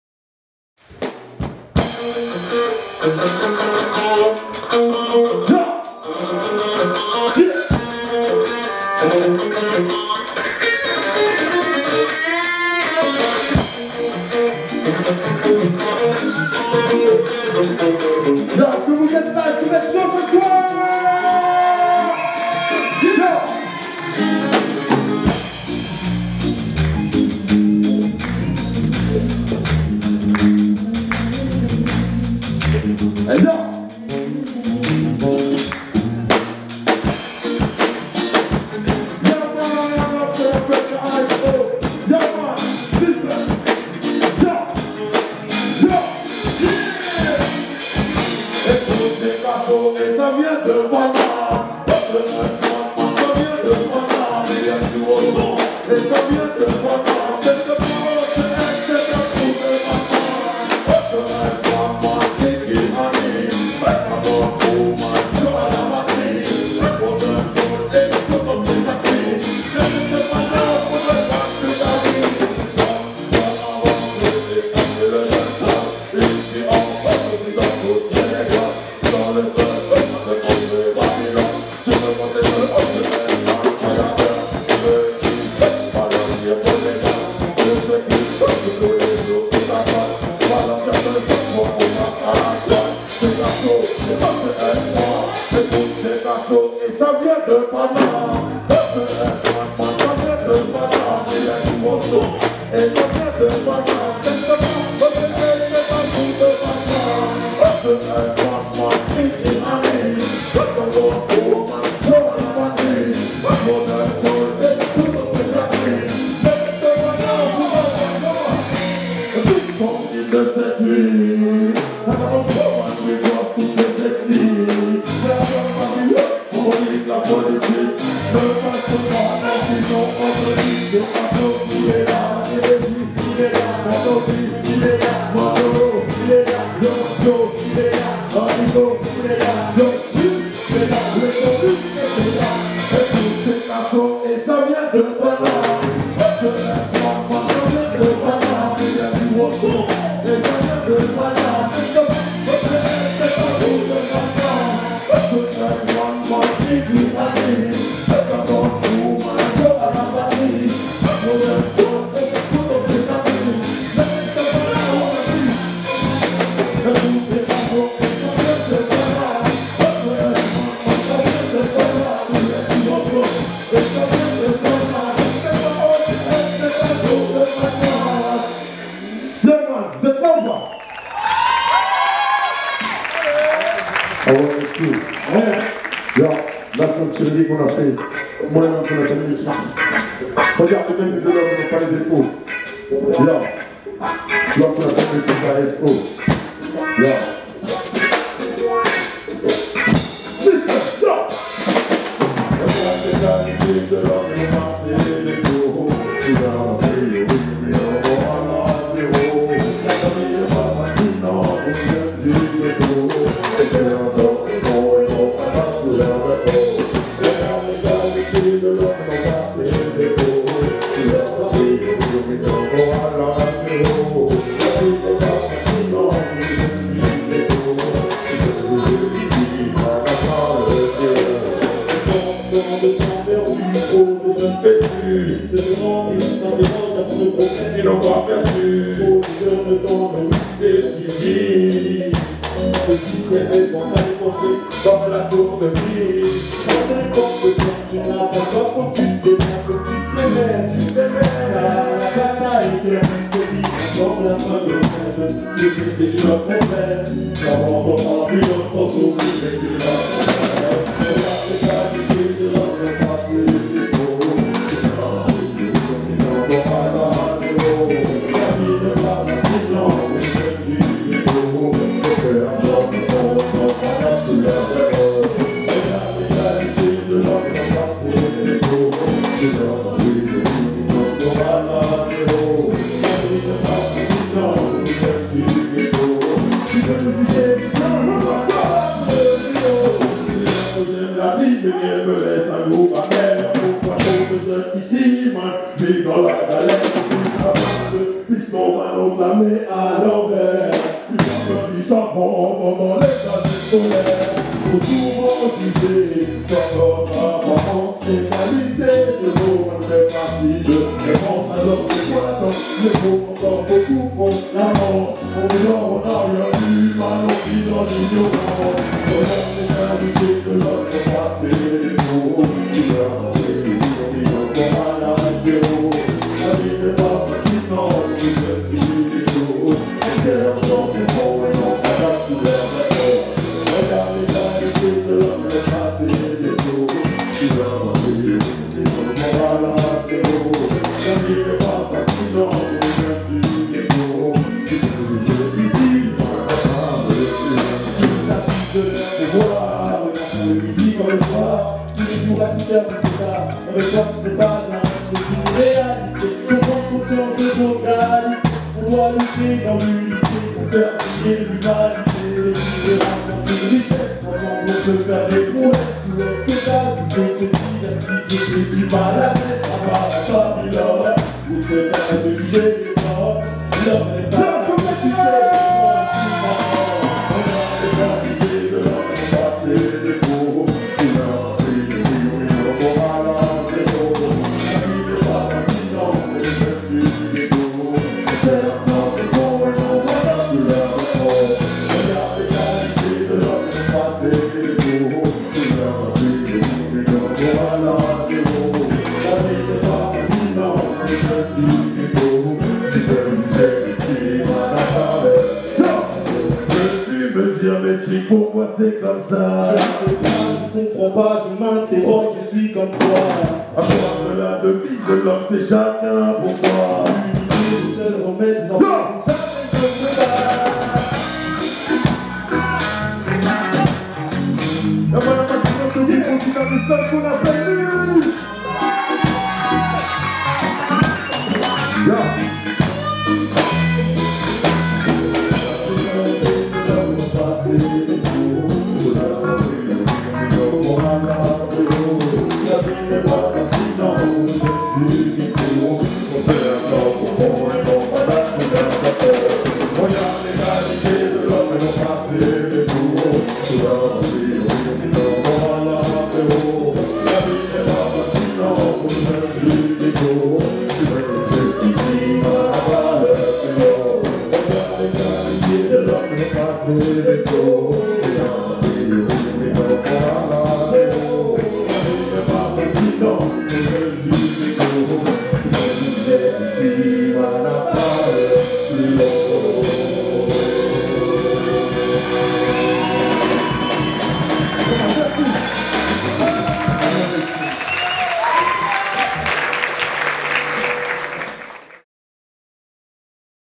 Kasö filmé par LC.FiLM au Festival REC
KASÖ, jeune chanteur des Yvelines qui arrive en force, originaire de Guyancourt, il a trouvé sa voie, dans le Reggae, Dance-hall où il excelle ...

monté par Audric GUEIDAN